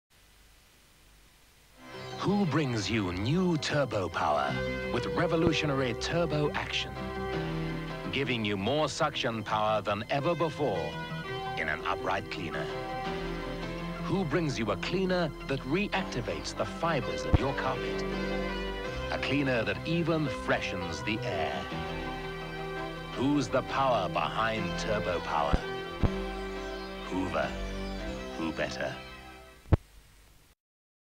Hoover Turbopower Upright Vacuum Cleaner TV Commercial 1984
This is a longer version of the Turbopower advert I uploaded a while ago. The very first version had a slightly different narration which stated the Hoover brand early on without asking the question, "Who brings you new Turbopower?"
This dates from 1984, the launch ad for the Turbopower was first aired in 1983.